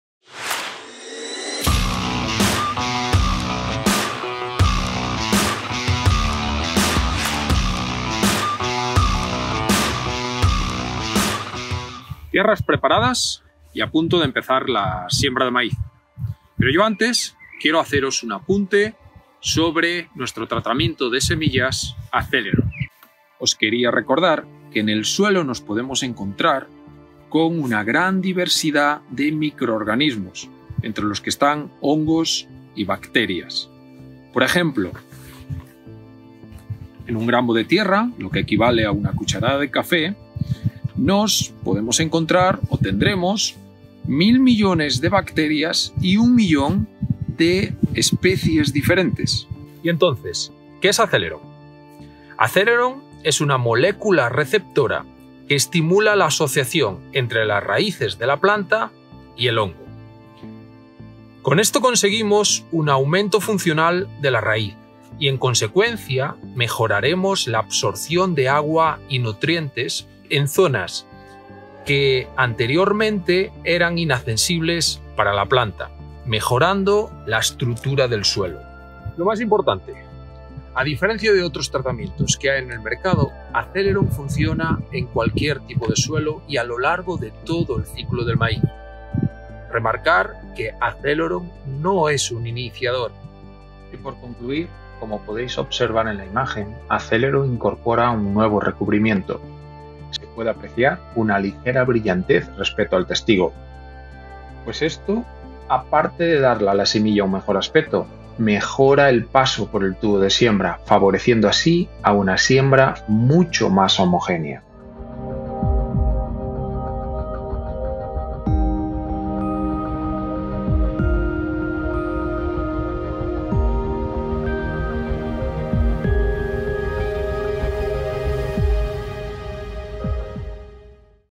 Acceleron® 🚀🌽 Maximiza tu producción desde el inicio
Las soluciones Acceleron® aplicadas a la semilla protegen tu cultivo especialmente durante los primeros 30 días, mejorando el vigor de la planta y la uniformidad del cultivo.